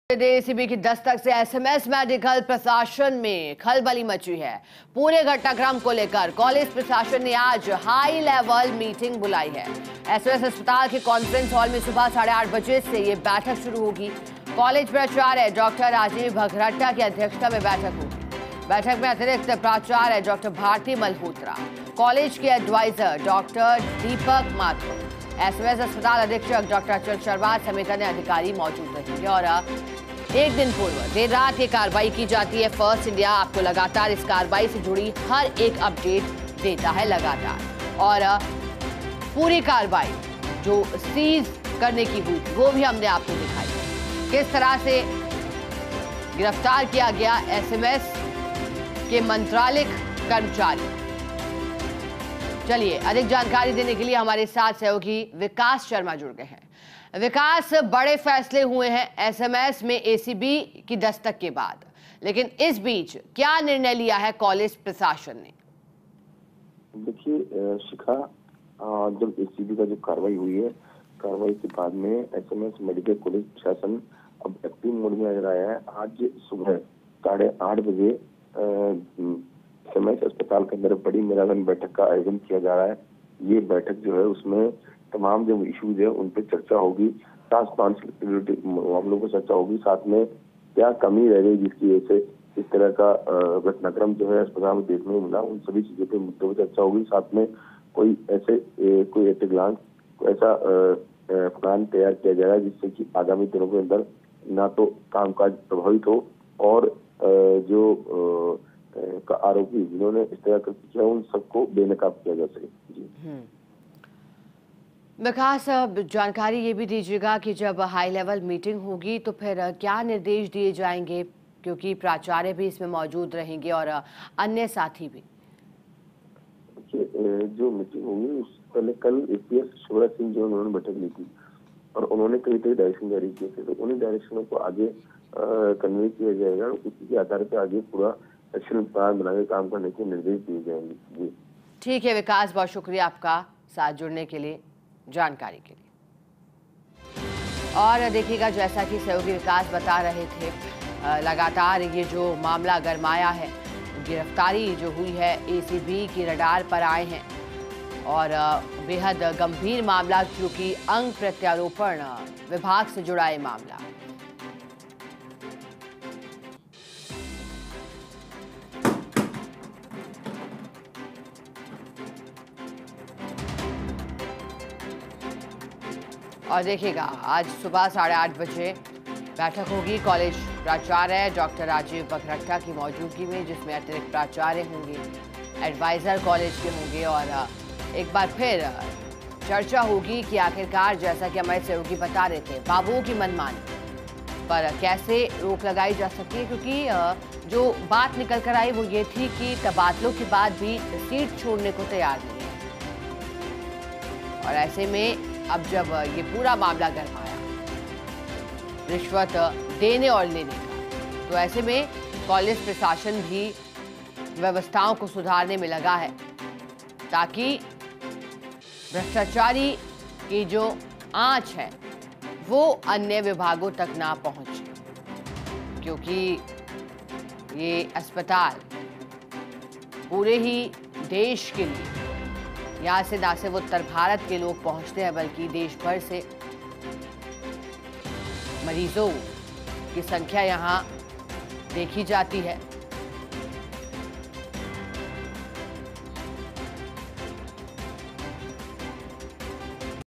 ACB की दस्तक से SMS मेडिकल कॉलेज प्रशासन में खलबली, आज बुलाई हाईलेवल मीटिंग | Rajasthan News
About this Video:

जयपुर,ACB की दस्तक से SMS मेडिकल कॉलेज प्रशासन में खलबली,पूरे घटनाक्रम को लेकर कॉलेज प्रशासन ने आज बुलाई हाईलेवल मीटिंग,SMS अस्पताल के कॉन्फ्रेंस हॉल में सुबह 8:30 बजे से शुरू होगी बैठक,कॉलेज प्राचार्य डॉ राजीव बगरहट्टा की अध्यक्षता में आयोजित होगी बैठक,बैठक में अतिरिक्त प्राचार्य डॉ. भारती मल्होत्रा, कॉलेज के एडवाइजर डॉ. दीपक माथुर ,SMS अस्पताल अधीक्षक डॉ. अचल शर्मा समेत अन्य अधिकारी रहेंगे मौजूद ,  | 02 April 2024